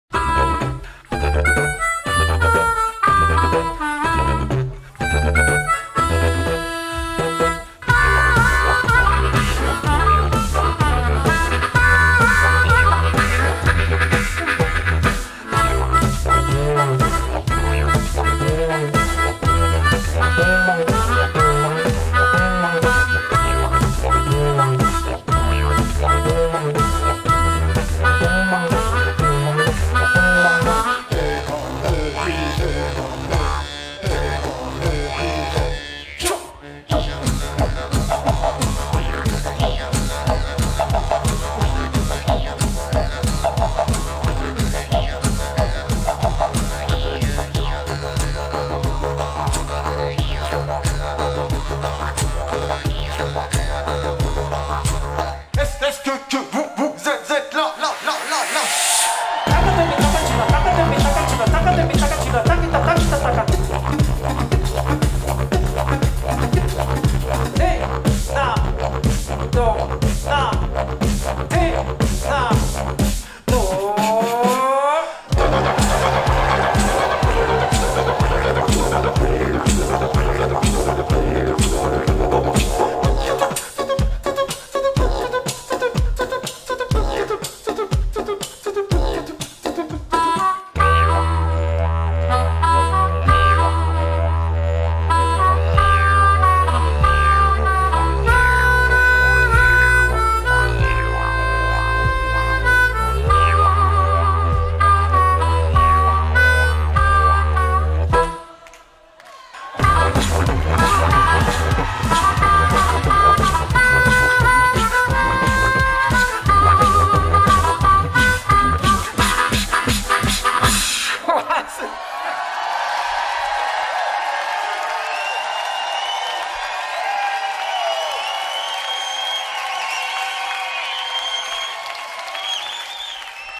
Live @ Rêve de l'aborigène 2024 - Underground Cosmic Didg
Teaser extract from Le Rêve de l'aborigène 2024 !
Danse Krump : Wrestler
Video shooting : Manu, Sam et les Rêveurs
Montage : Zalem